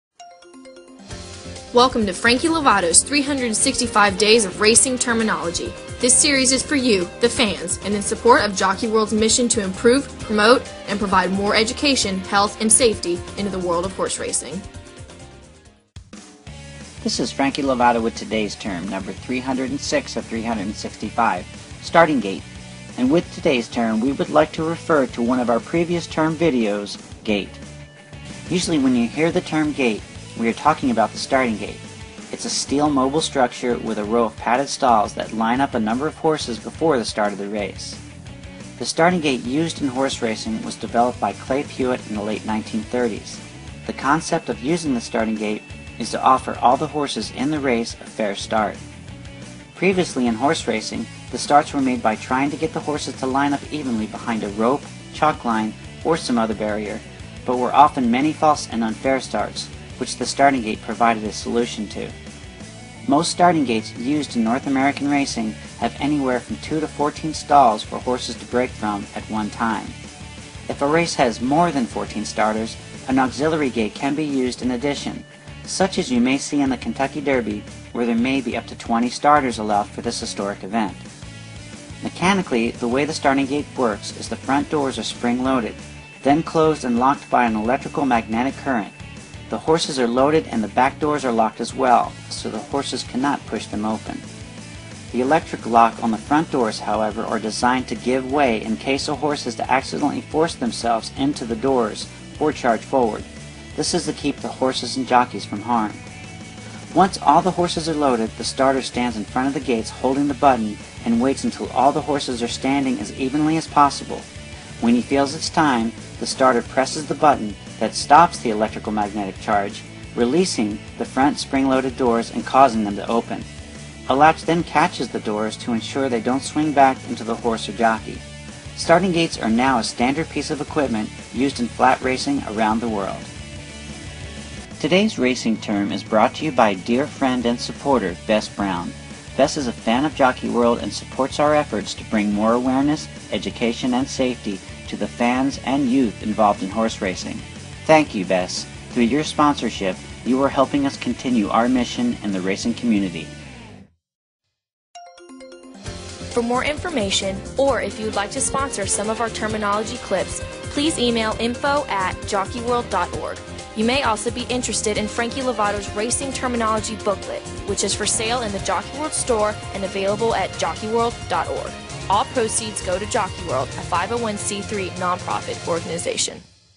Racing Term #306 "Starting Gate" of Frankie Lovato's 365 Days of Terminology
Retired Eclipse Award-winning Jockey Frankie Lovato, Jr takes on the challenge of creating 365 videos for each day of the 2013 year. These are all definitions taken from his racing terminology booklet "Frank Lovato, Jr's Racing Terminology". and proceeds support Jockey World, a 501c3 non-profit organization that's mission is horse racing education.